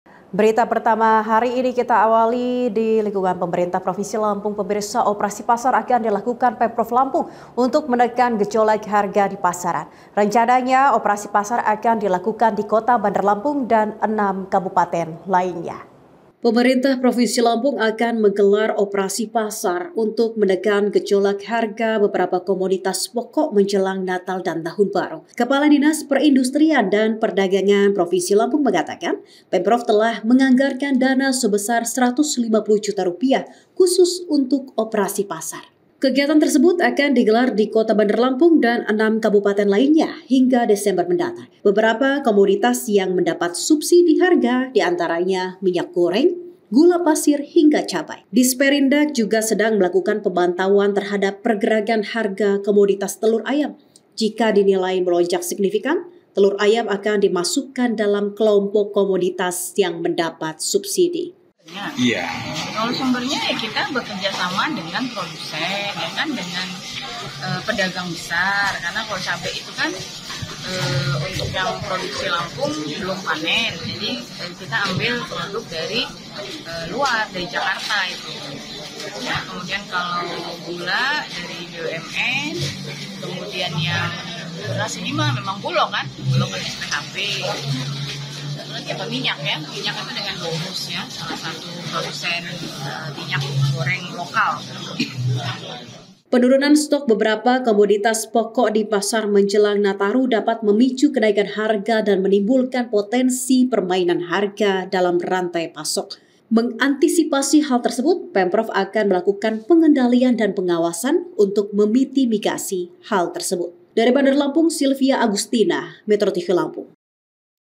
Tekan Gejolak Harga Pemprov Lampung Gelar Operasi Pasar
Operasi pasar akan dilakukan Pemprov Lampung untuk menekan gejolak harga di pasaran. Rencananya operasi pasar akan di lakukan di Kota Bandar Lampung , dan enam kabupaten lainnya.